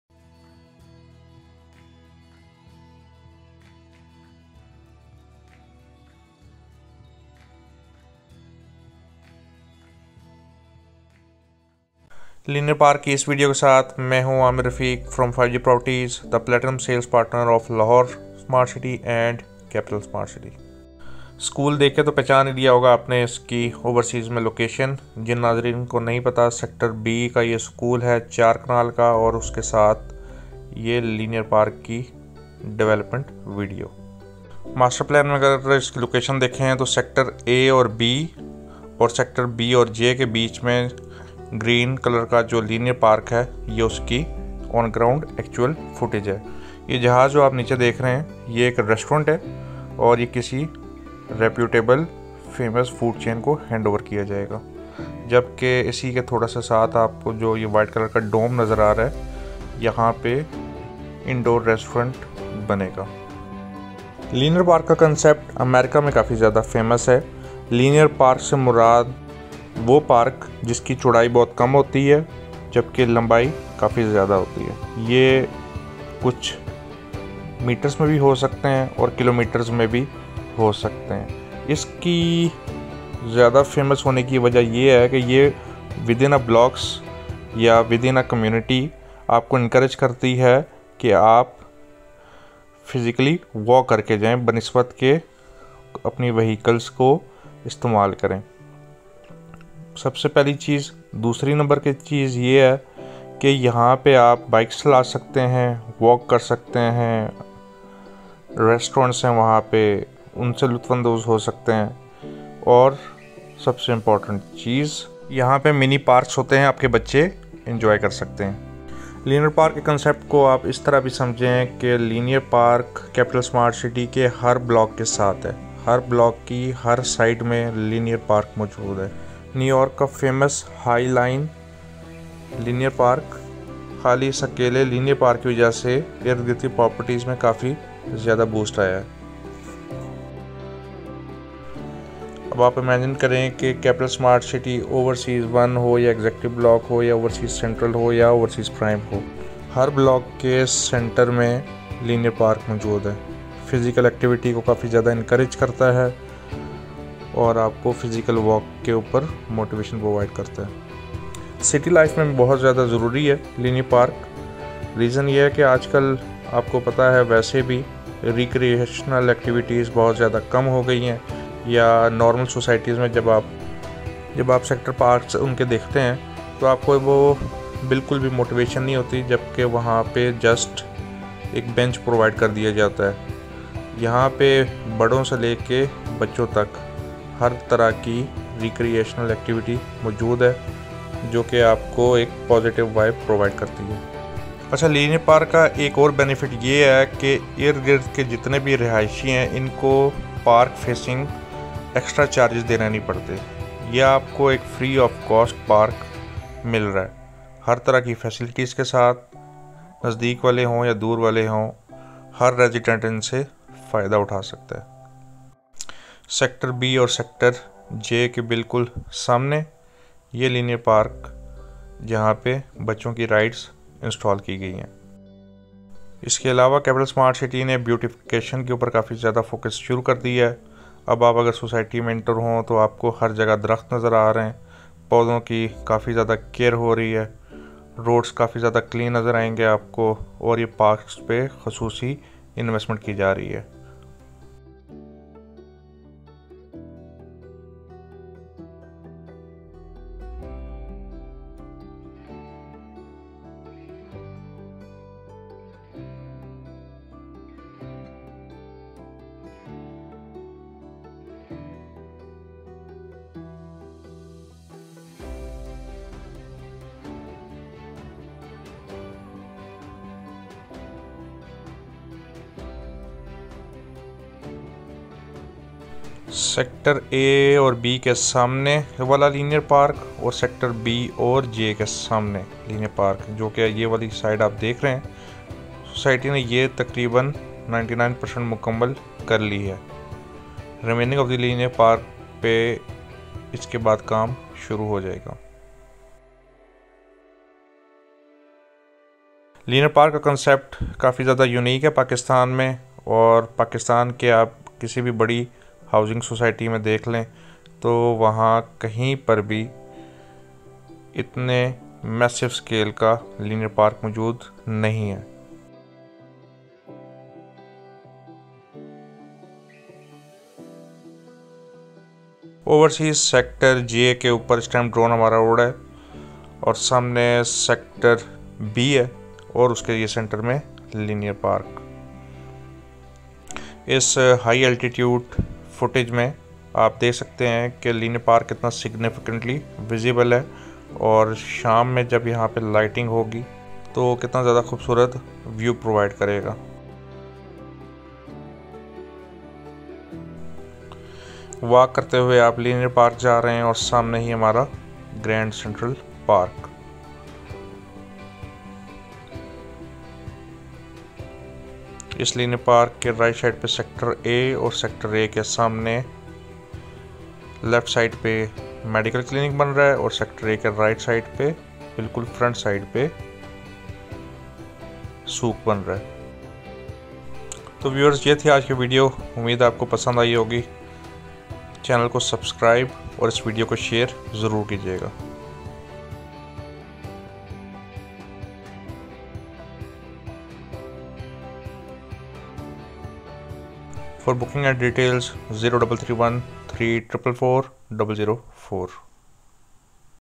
Linear Parks - Concept | Development Update | Capital smart city Islamabad
recorded on
20th December 2021

In just one month rapid development has shown progress and can be seen significant changes from my old video.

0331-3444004